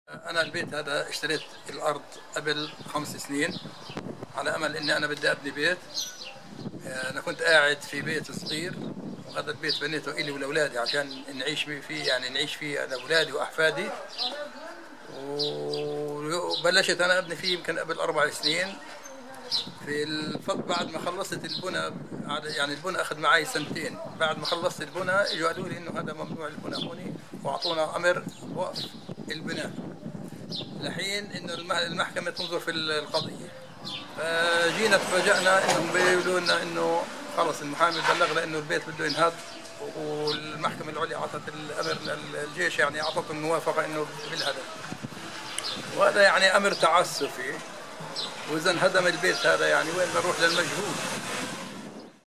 Ghaleb Abu Hadwan on demolition risk
Ghaleb Abu Hadwan, a 63-year-old, alking about the risk of demolition of his home is Sur Bahir (Jerusalem), July 2019